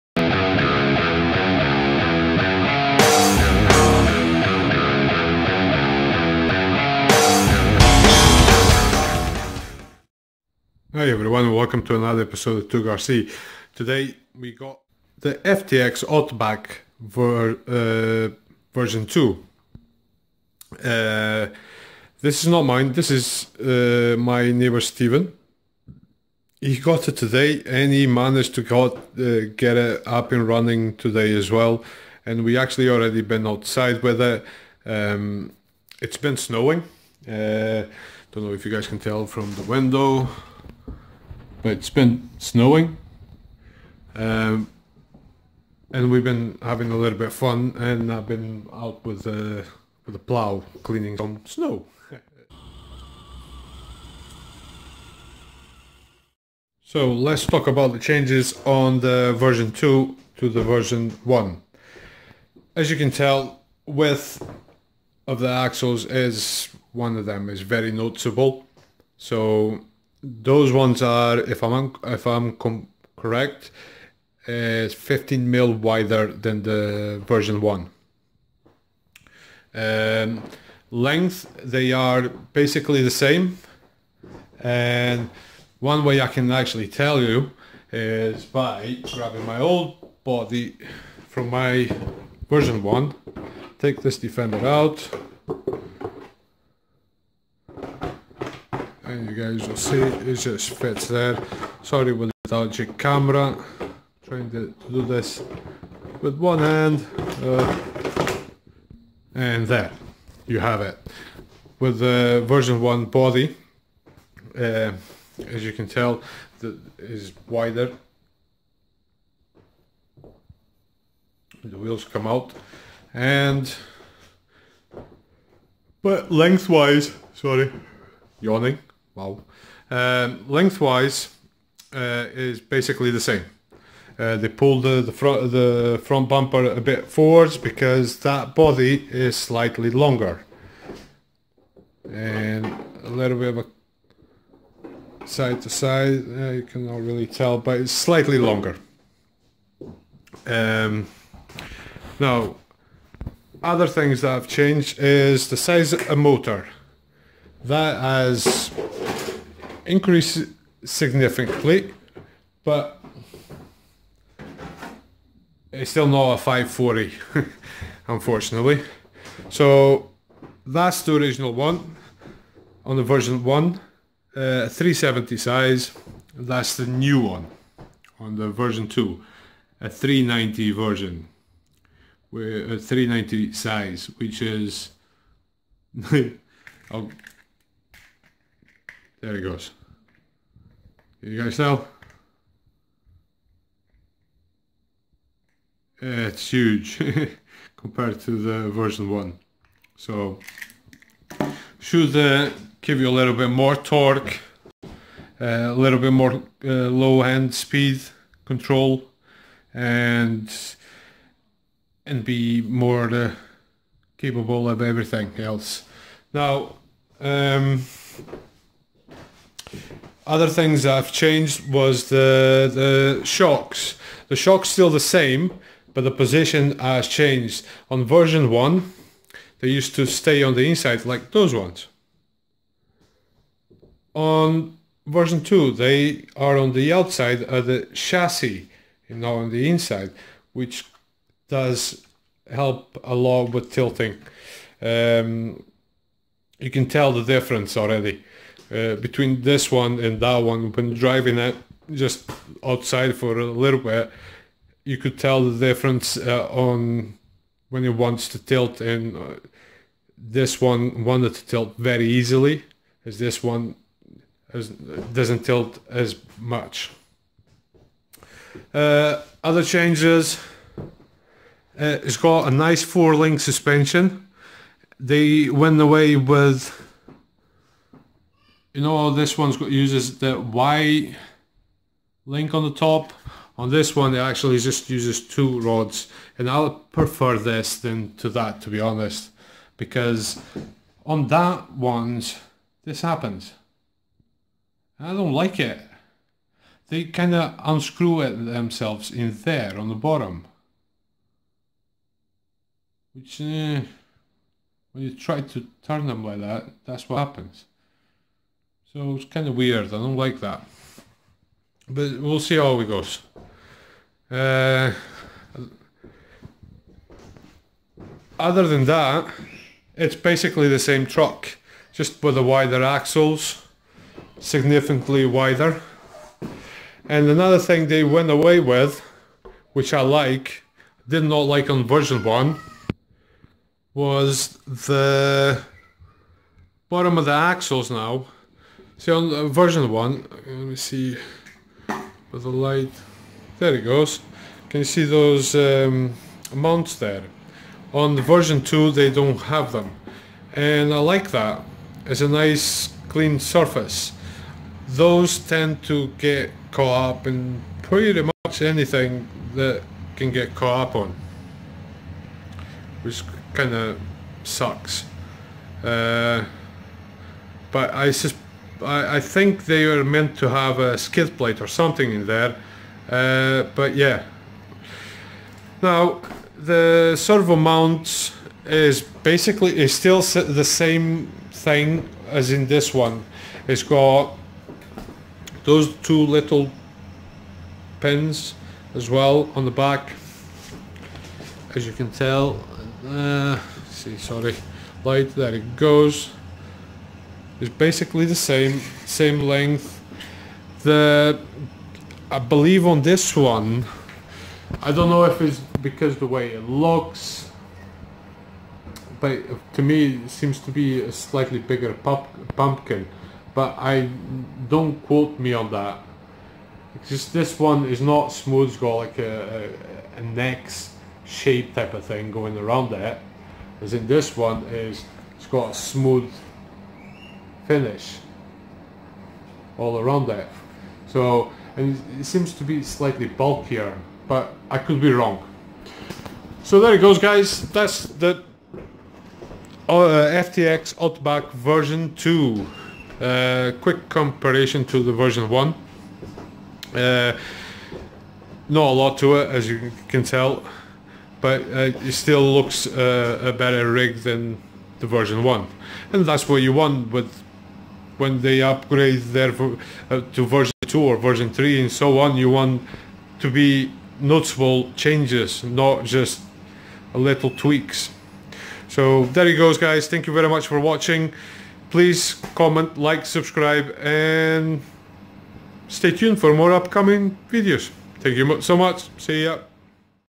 Ftx Outback 2 & Ftx Outback (what's different)
Just a quick look at the new version of the Ftx Outback